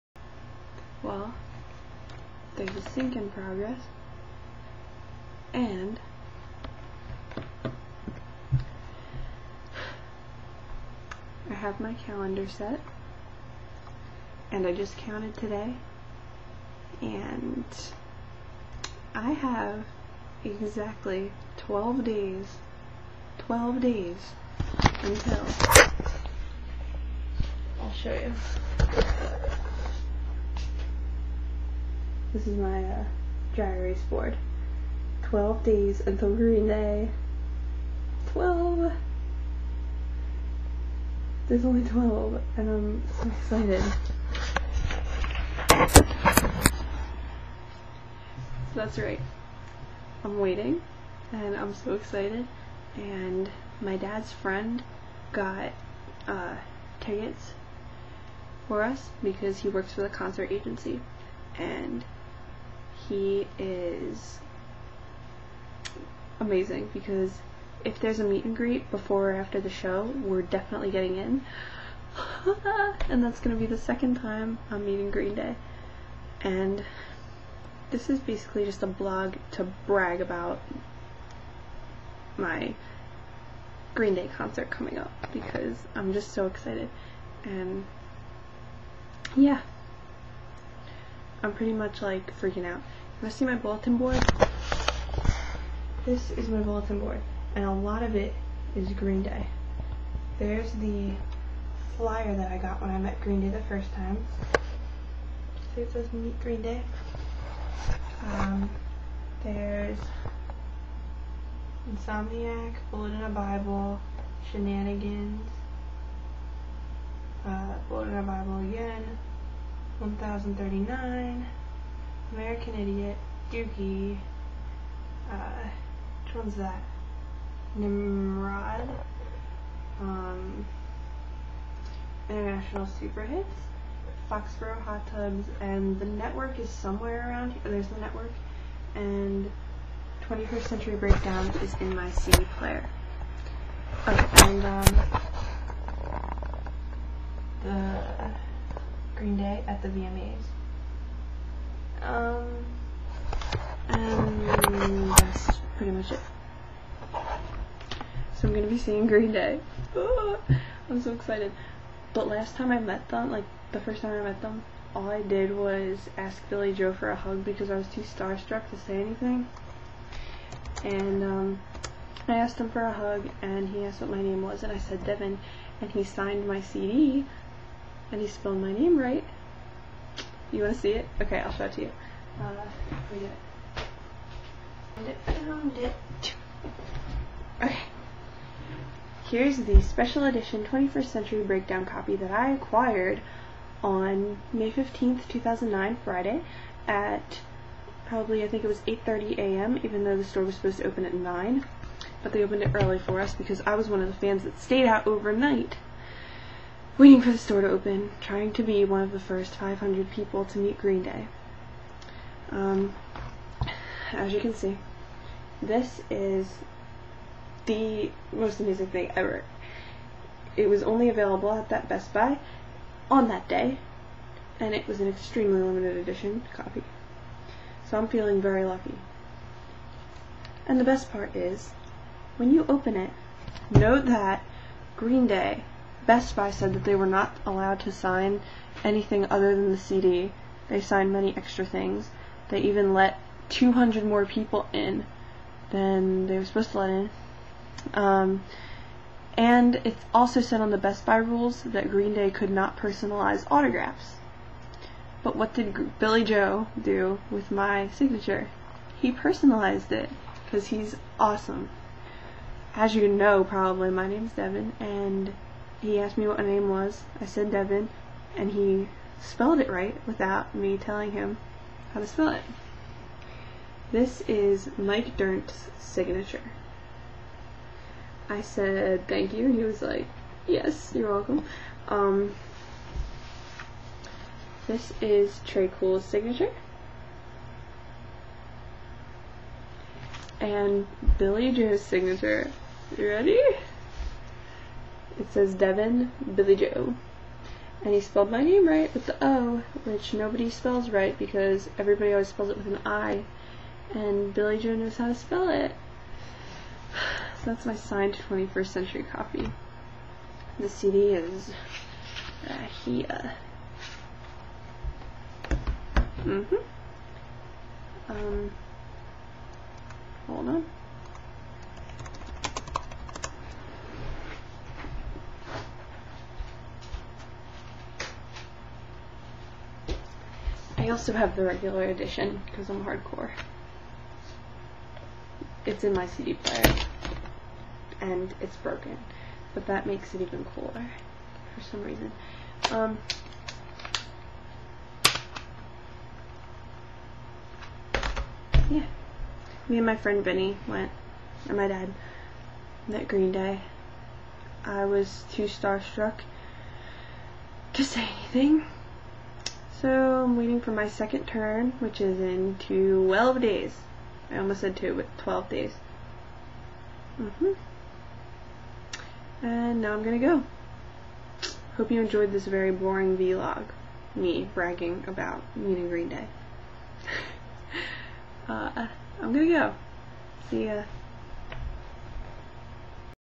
blog about meeting green day and the concert!!
well now i actually went and it was AMAZINGLY INCREDIBLE BEYOND BELIEFF!